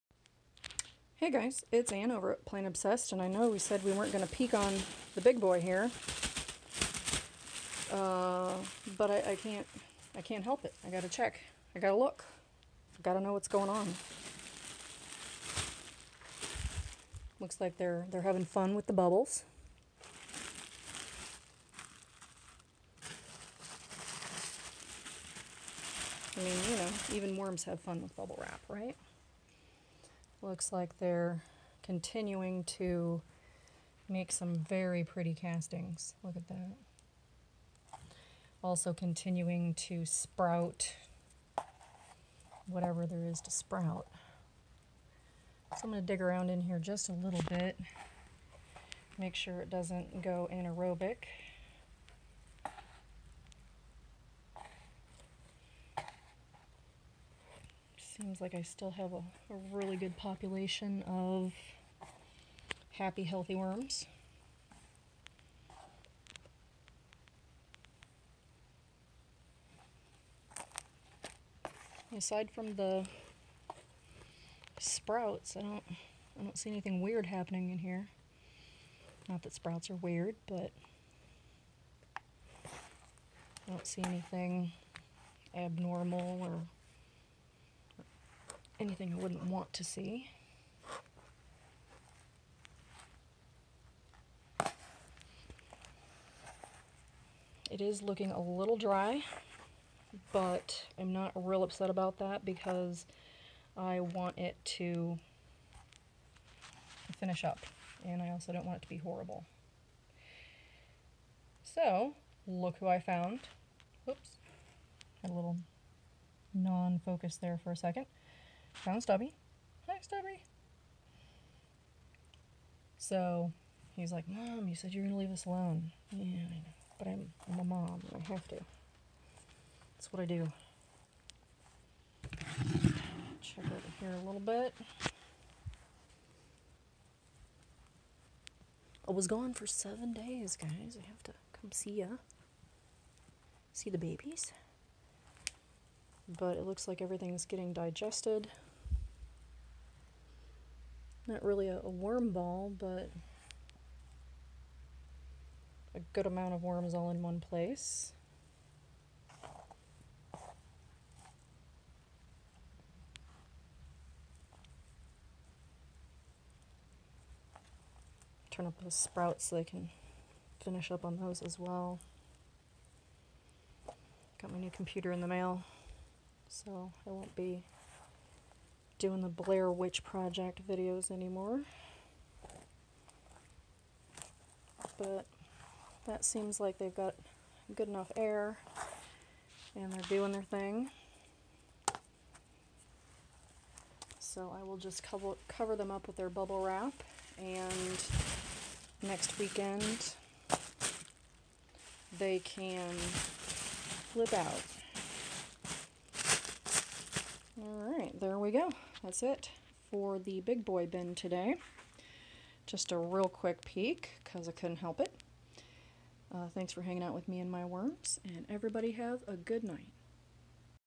Sneekpeek at BIGBOY the 55 gallon worm bin
I couldn't wait. I had to peek after being gone for 7 days.